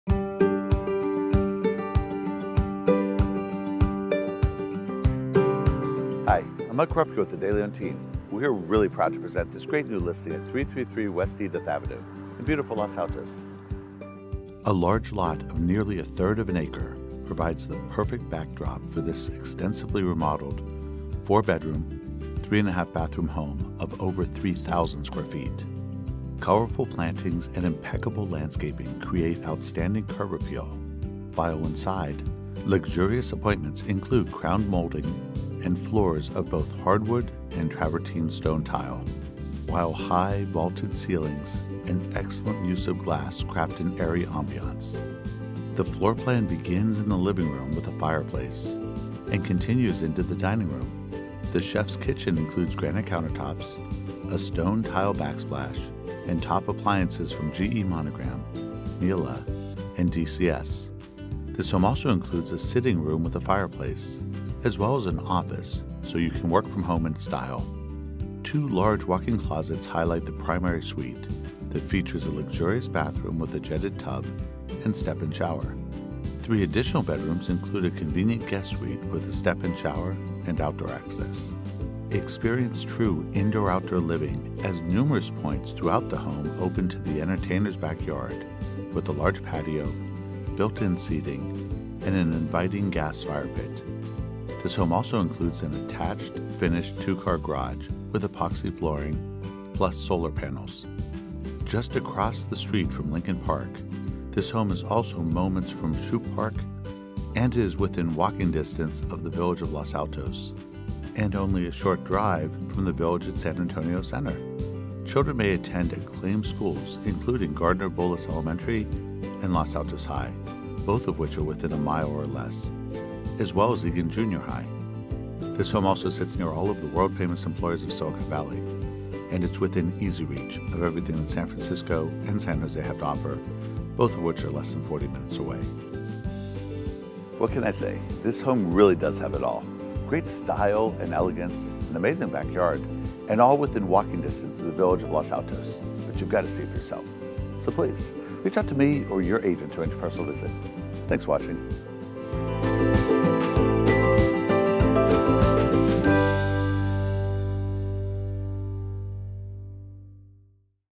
333 W Edith Avenue, Los Altos | DeLeon Realty Listing
4 Beds, 3.5 Baths | 3,048 Sq. Ft. House | 13,780 Sq. Ft. Lot
Open House On Saturday 6/11 & Sunday 6/12, 1:30 PM - 4:30 PM

BEAUTIFULLY REMODELED AND CLOSE TO TOP SCHOOLS
A large lot of nearly a third of an acre provides the perfect backdrop for this extensively remodeled 4-bedroom, 3.5-bathroom home of over 3,000 square feet. Colorful plantings and impeccable landscaping create outstanding curb appeal, and inside, luxurious appointments include crown molding and floors of both hardwood and stone tile, while high, vaulted ceilings and excellent use of glass craft an airy ambiance. The traditional floorplan includes the expansive living room with a brick fireplace, the chef’s kitchen with appliances from Miele, DCS, and Monogram, the inviting sitting room, and the office that allows you to work from home in style. Comfortable bedrooms are highlighted by the primary suite with 2 large walk-in closets, plus the convenient guest suite. Enjoy true indoor/outdoor living as multiple points throughout the home open to the entertainer’s backyard with a large patio, gas fire pit, and built-in seating. Plus, this home enjoys a finished 2-car garage, as well as solar panels. Just across the street from Lincoln Park, this home is also moments from Shoup Park, and minutes to the amenities of downtown Los Altos, with top-ranked schools Gardner Bullis Elementary and Los Altos High within a mile or less.